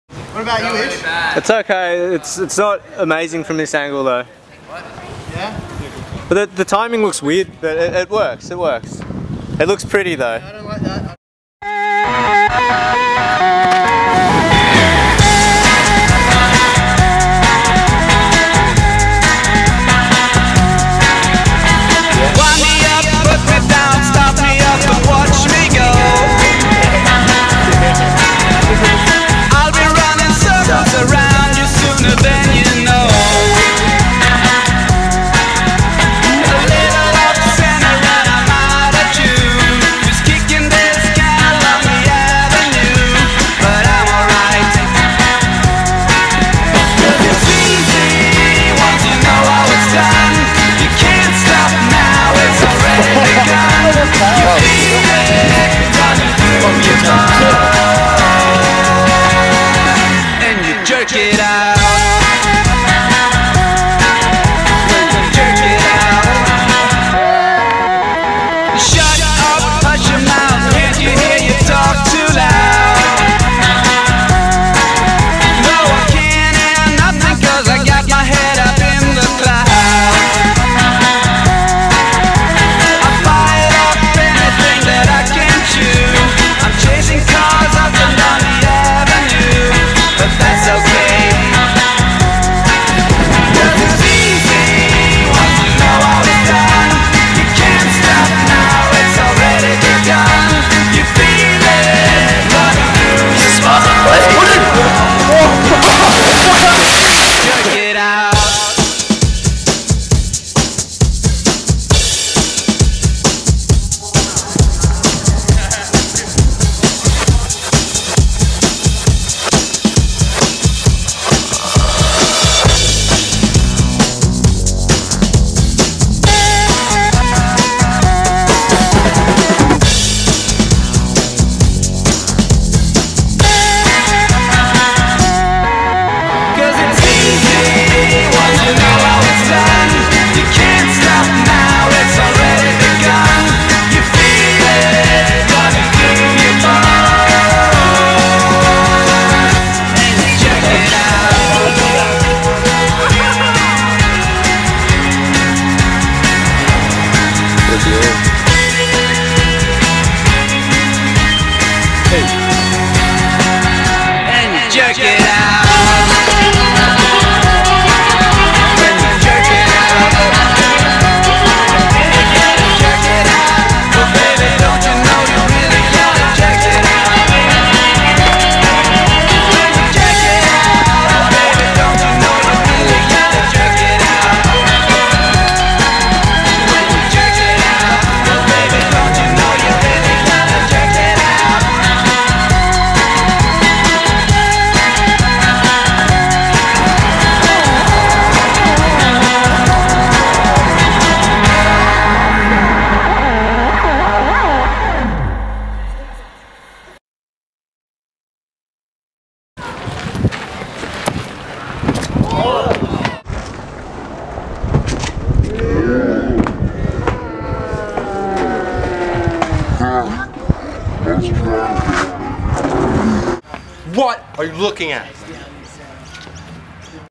UNSW session
Sydney People in UNSW jumpin around, amongst other places.

Edited by Dominic Di Tommaso